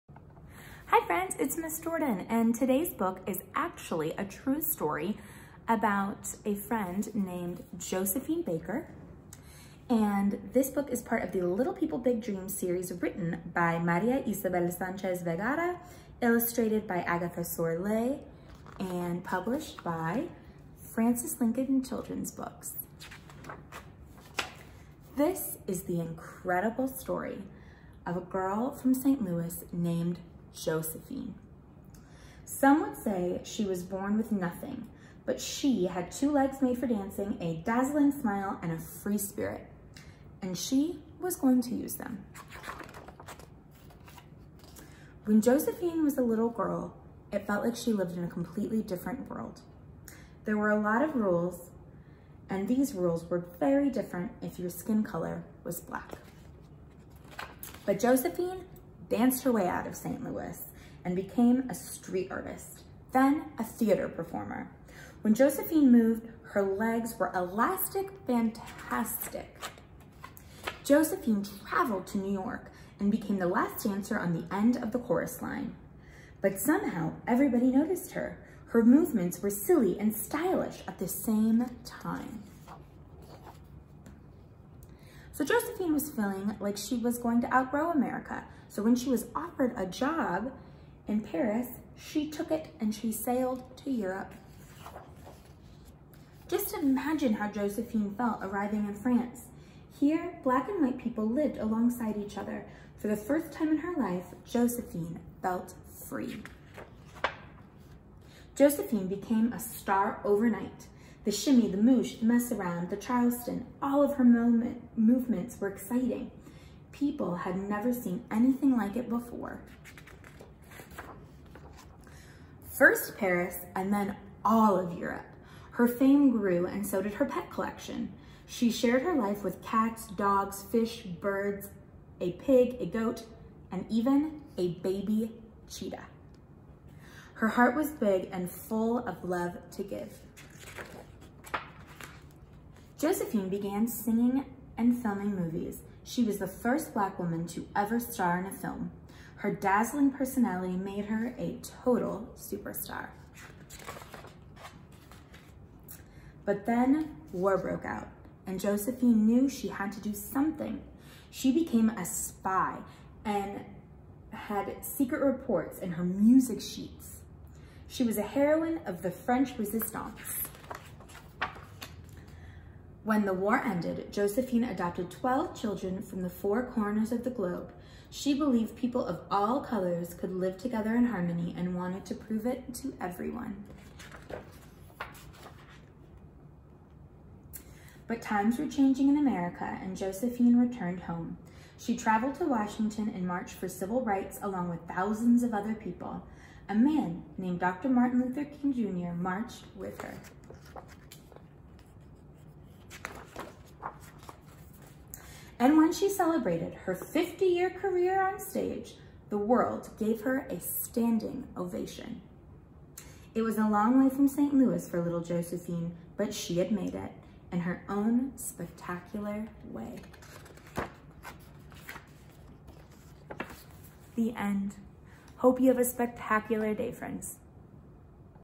Miss Jordan reads "Josephine Baker"
Josephine Baker grew up in in the middle of the United States, but she grew up to be a world famous entertainer, spy and civil right activist. This true story by by Maria Isabel Sanchez Vegara and illustrated by Agathe Sorlet is part of the Little People, Big Dreams series from Frances Lincoln Children's Books.